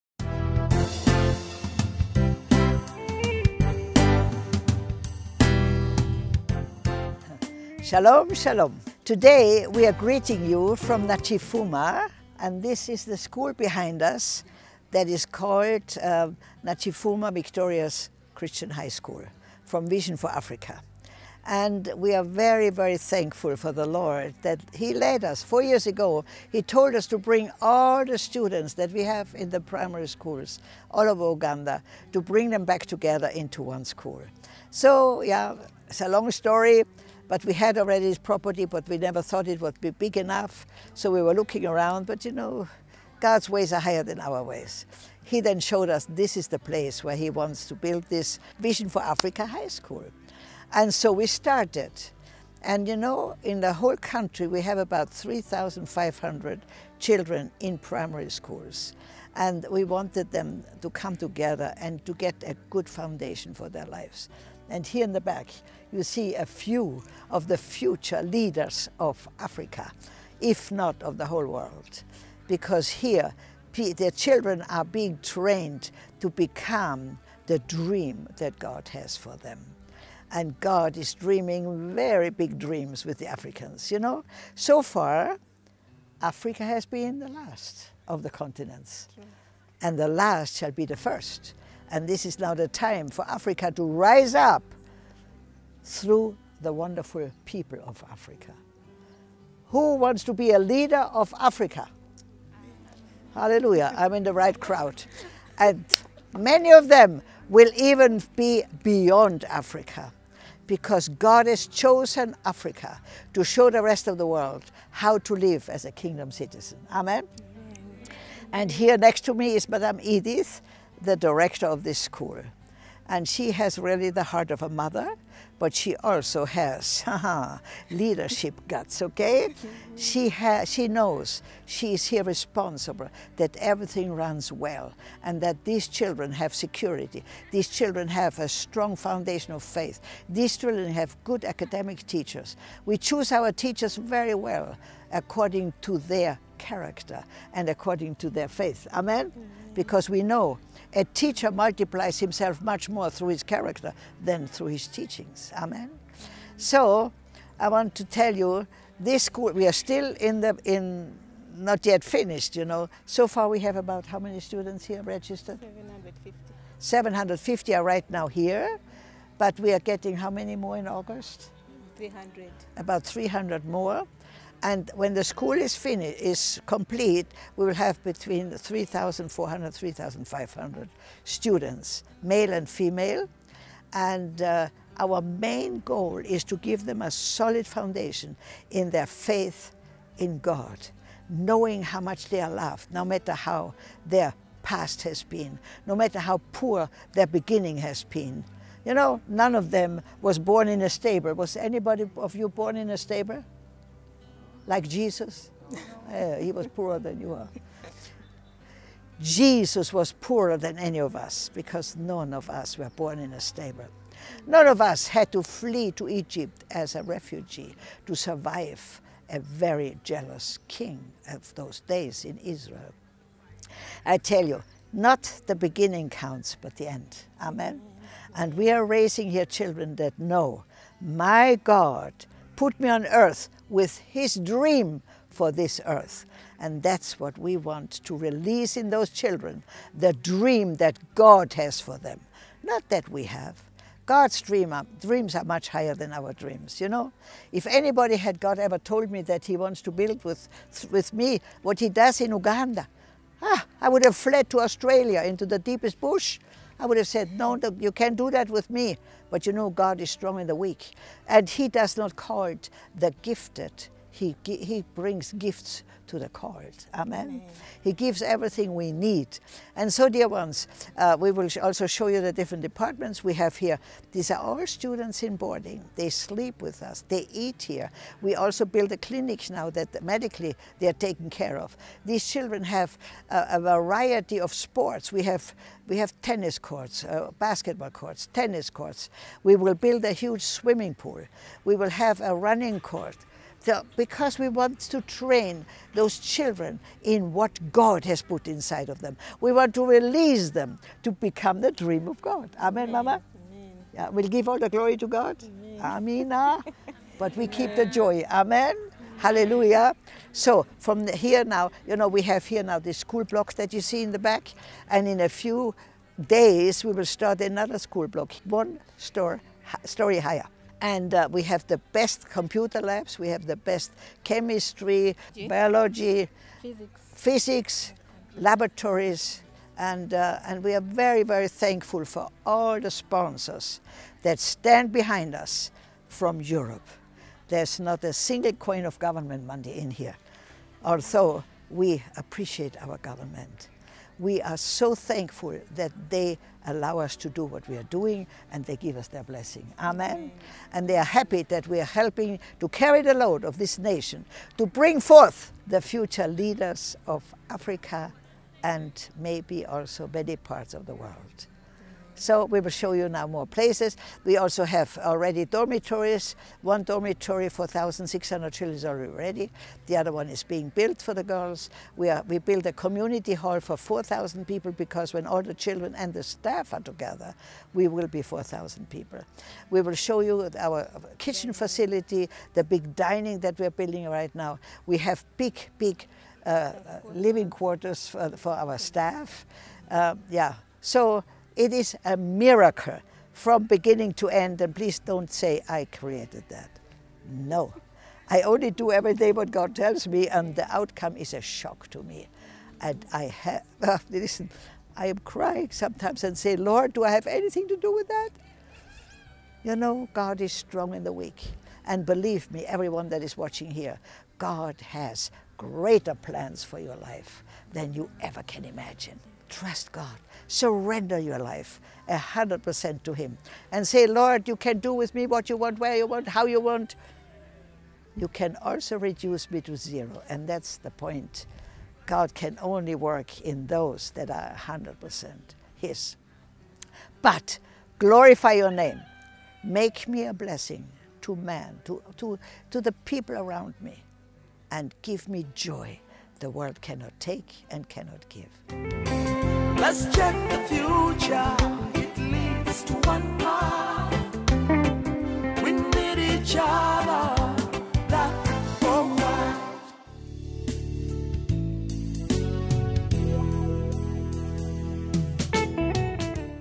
Vision for Africa Intl. High School Nakifuma - Image film (English HD)
Insights and updates on the High School in Nakifuma, Uganda of Vision for Africa Intl. (06/2021)